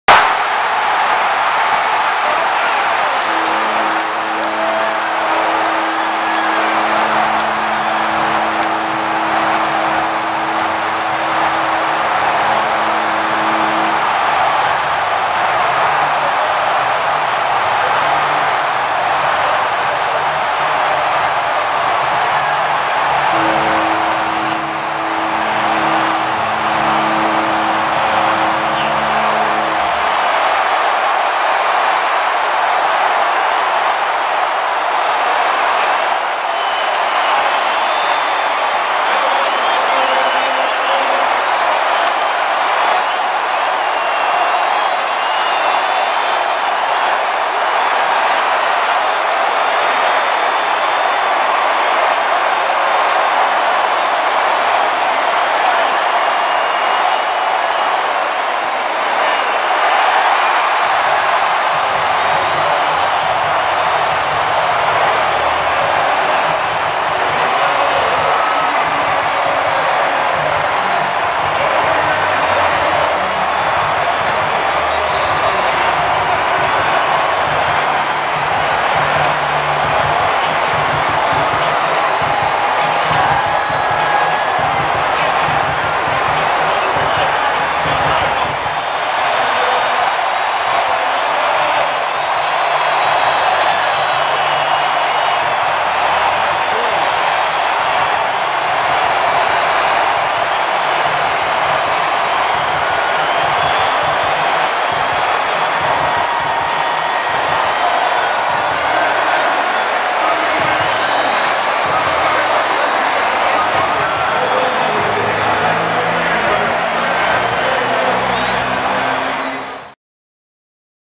Reaction to NaVorro Bowman pick 6 during last game at Candlestick Park
Here's what it was like on the field as Bowman ran down the sideline and scored the touchdown that clinched a win for the San Francisco 49ers in their last regular season game at Candlestick Park.